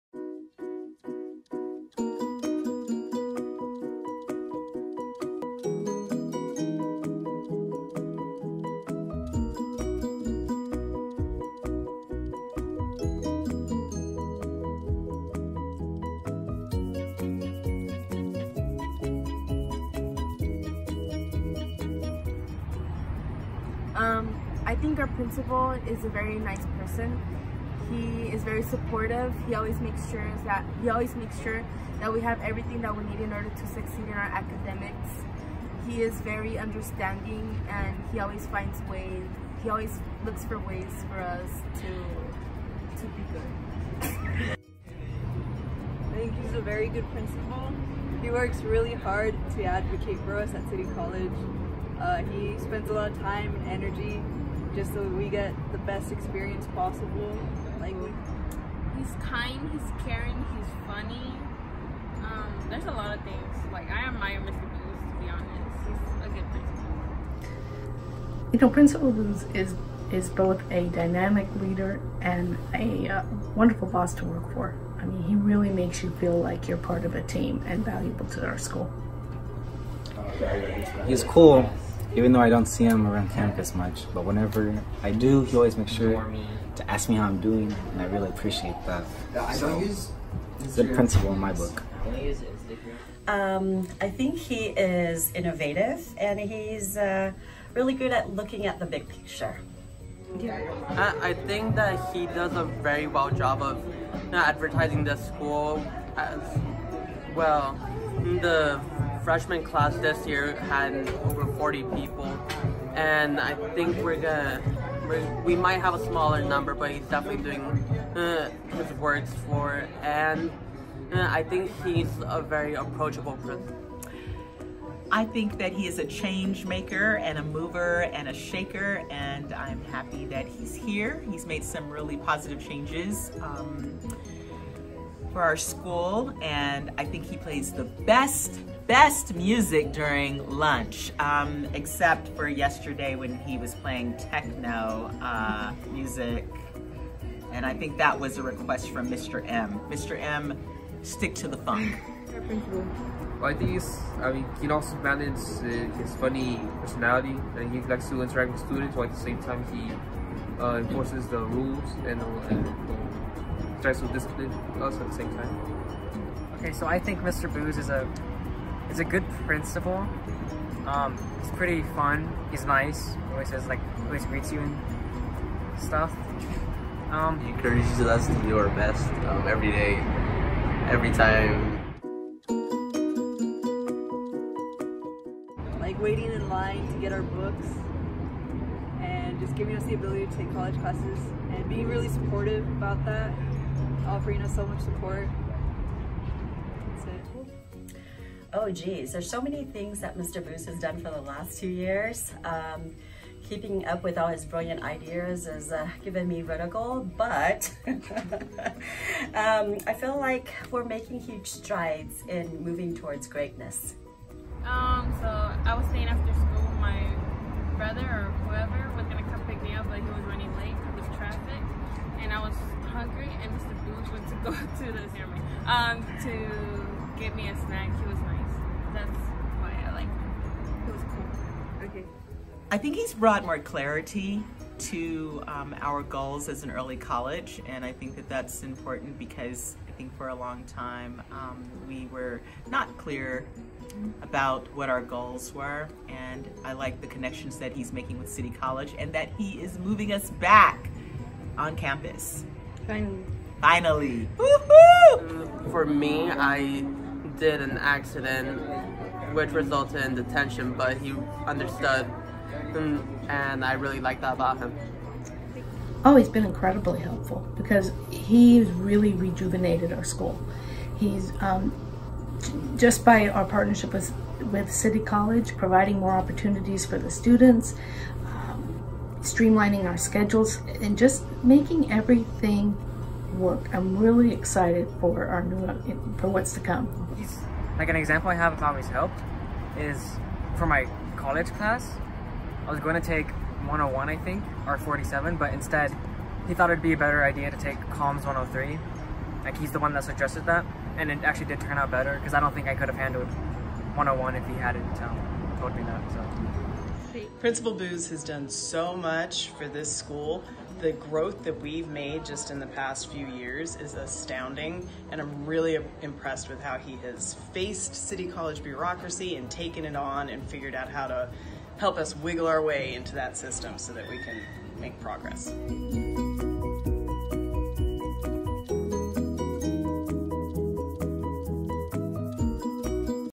Principals Appreciation Day - East Village Middle College High School
A video to show our appreciation for our wonderful principal, Mr.Booz from East Village Middle College High School in San Diego.